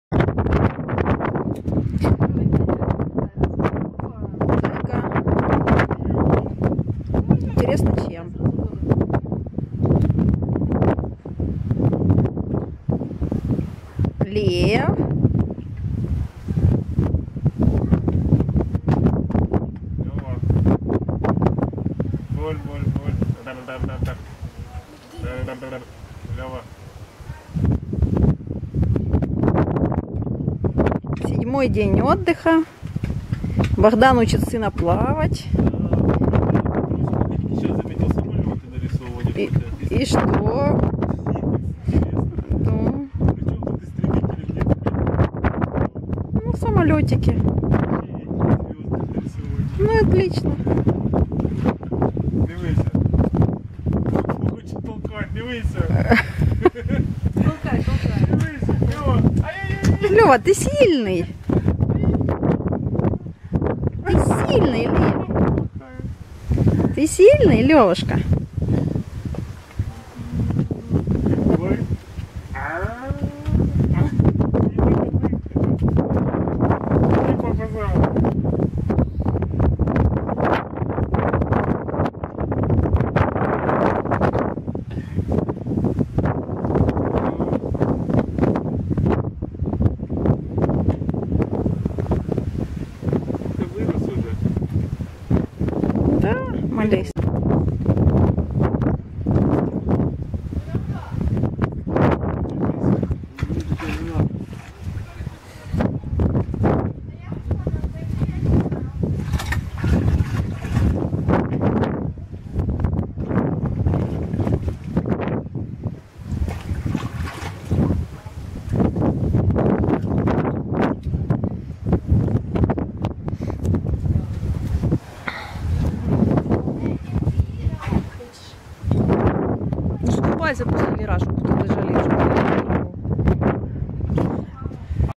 VLOG Малыш Лев едет и отдыхает в Кирилловке VLOG Little Leo rides and rests in Kirillovka
Малыш Лев с родителями на авто из днепра выехал на 7 дней на отдых в Кирилловку. В Кирилловке семья остановилась на косе Пересыпь на базе отдыха "Золотое Руно". 7 дней провели в номере люкс. На территории базы был бассейн, так что шторм осенью не помешал поплавать в морской воде. Также было кафе с домашней кухней и магазин с продуктами. Сентябрь был теплым.